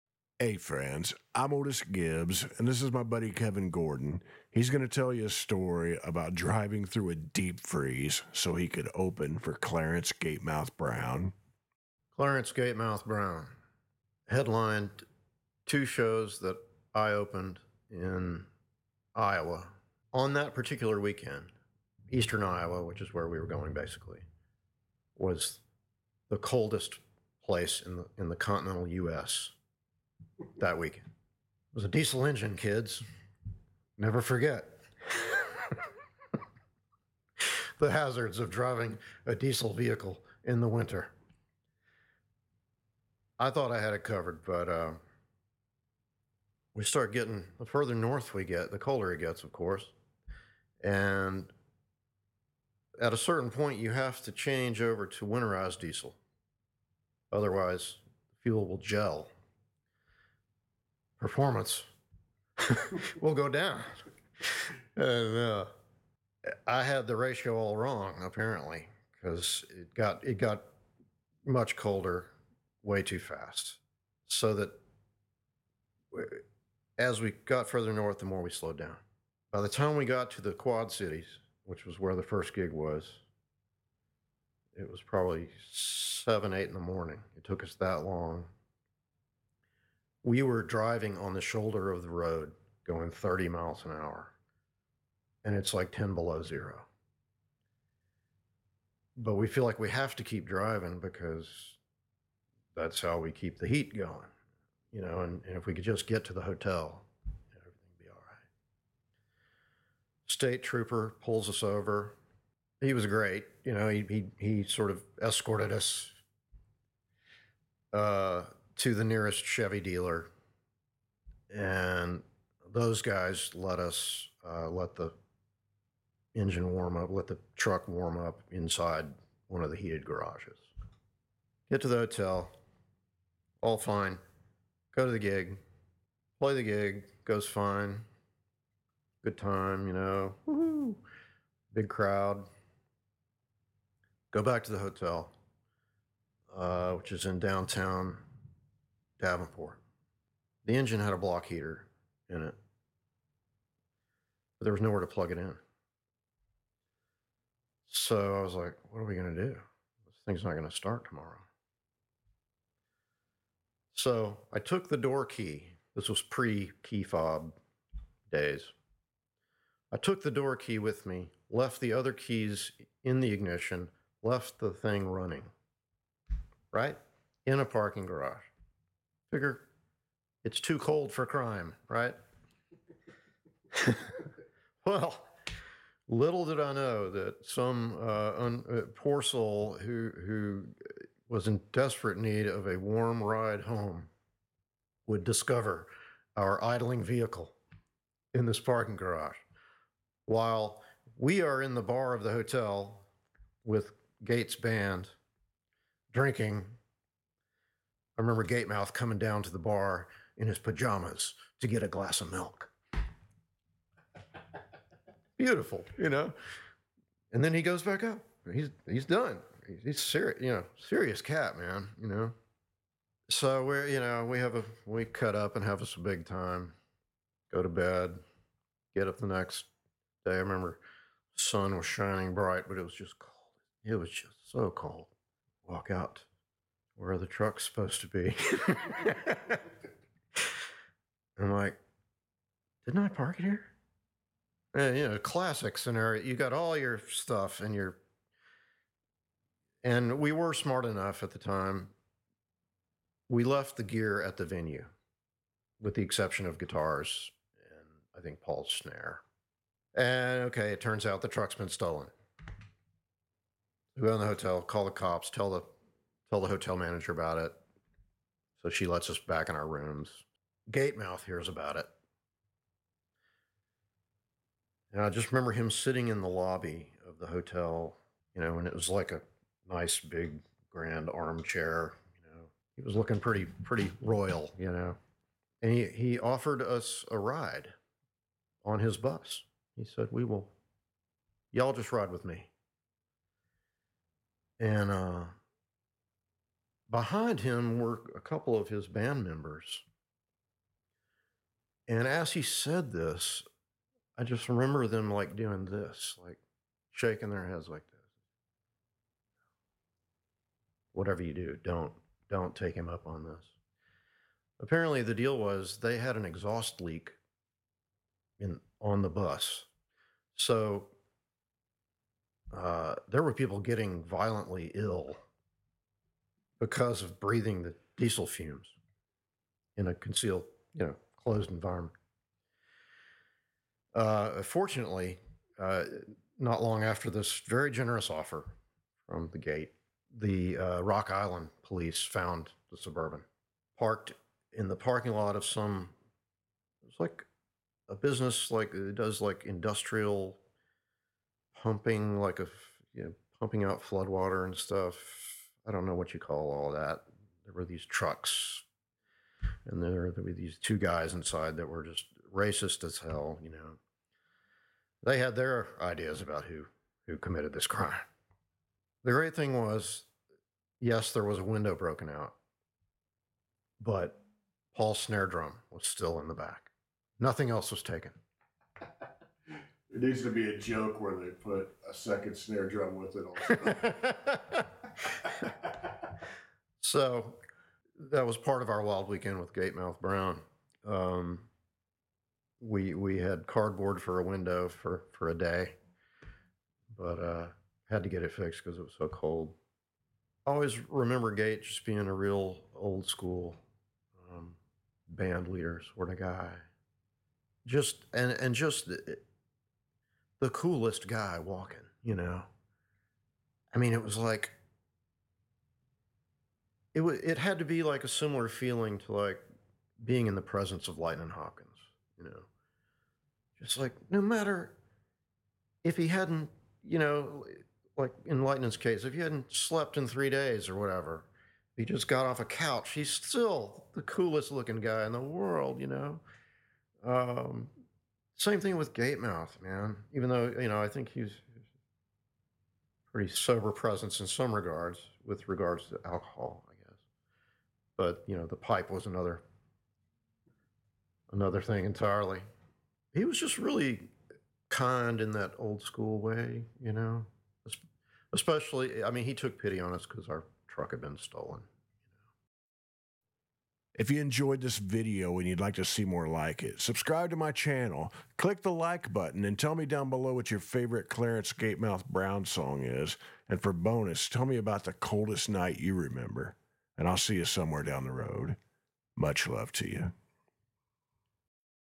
Clarence Gatemouth Brown -Deep Freezes and Stolen Trucks
Kevin Gordon talks about the things he had to endure to open for the great, Clarence Gatemouth Brown.